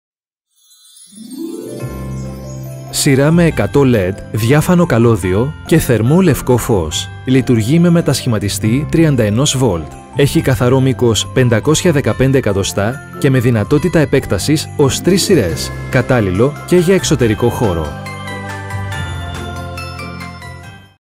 Επεκτεινόμενα 100 LED θερμό λευκό, διάφανο καλώδιο / Connectable 100LED warm white, transparent wire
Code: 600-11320
Σειρά με 100 LED διάφανο καλώδιο και θερμό λευκό φωτισμό. Λειτουργεί με μετασχηματιστή 31V. Έχει καθαρό μήκος 515 cm και με δυνατότητα επέκτασης ως 3 σειρές. Κατάλληλο και για εξωτερικό χώρο.
Line, 100 LED 3mm, 31V, connector until 3, transparent wire, warm white LED per 5cm, IP44